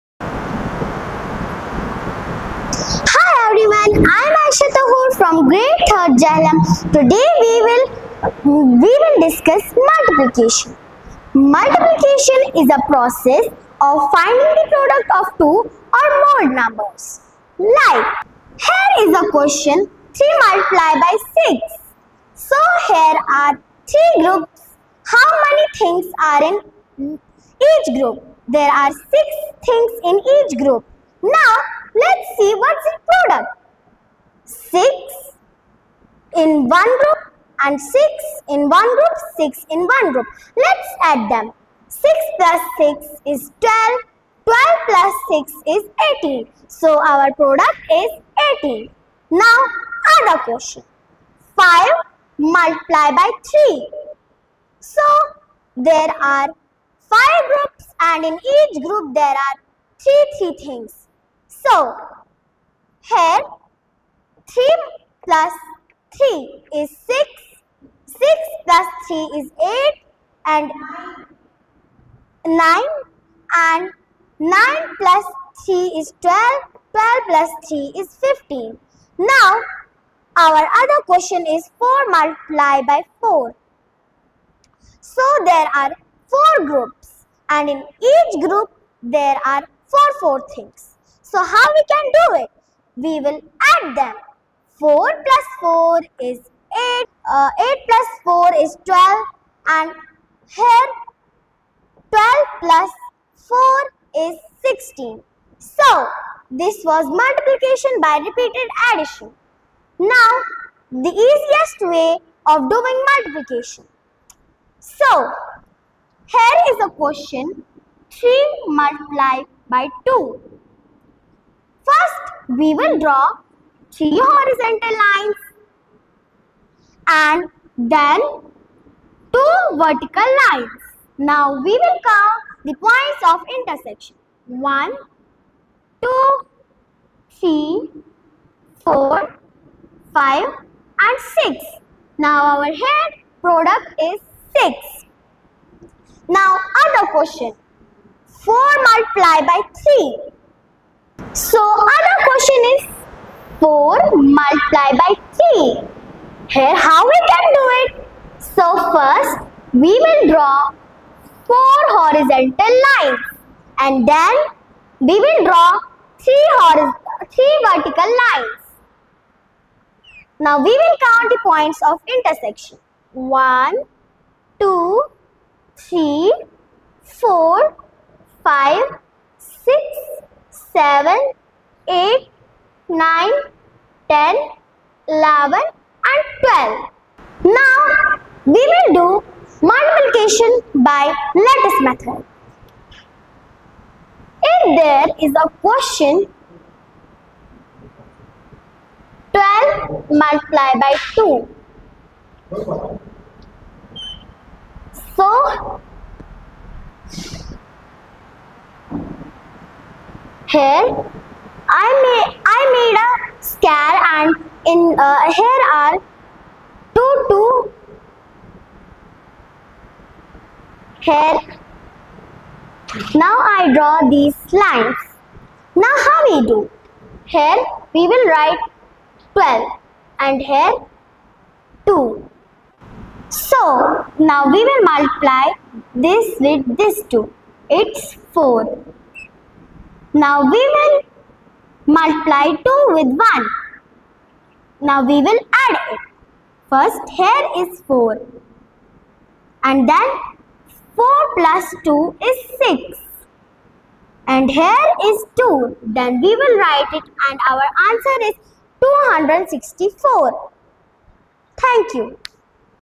Cute Educator of BOMIS Pampore
Cute Educator of BOMIS Pampore Teaching Multiplication